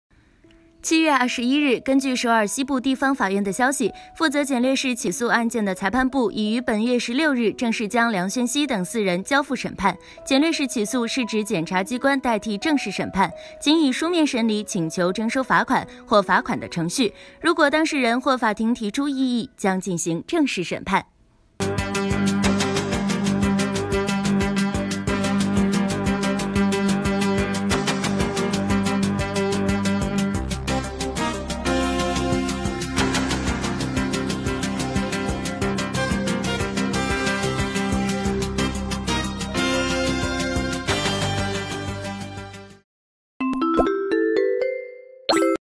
梁鉉錫賭博案將接受正式審判 涉嫌其他案件不起訴 審判結果成焦點！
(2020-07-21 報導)